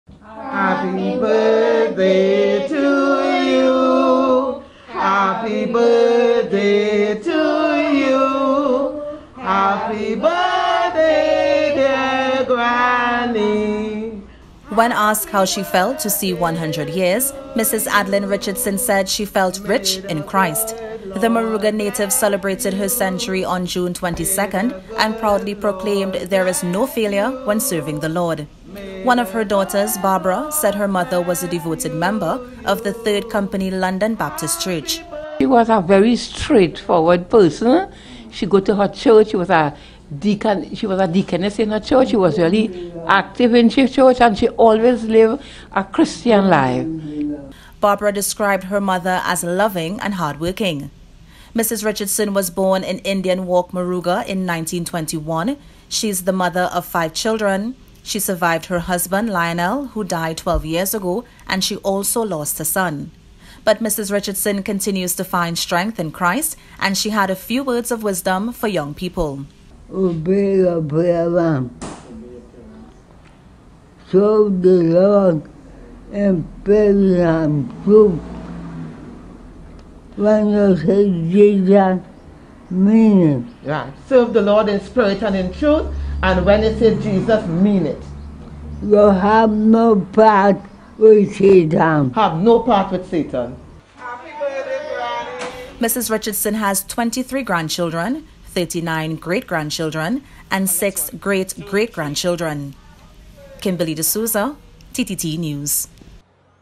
Mrs. Adlin Richardson Turns 100
Devotion to Jesus Christ is what T&T's most recent centenarian believes has kept her alive for 100 years.

Former Deaconess Mrs. Adlin Richardson celebrated her century on June 22nd, and she admitted she spent her life serving the Lord.

Cameraman Phil Britton spoke to the centenarian and her relatives, and Kimberly De Souza tells us more.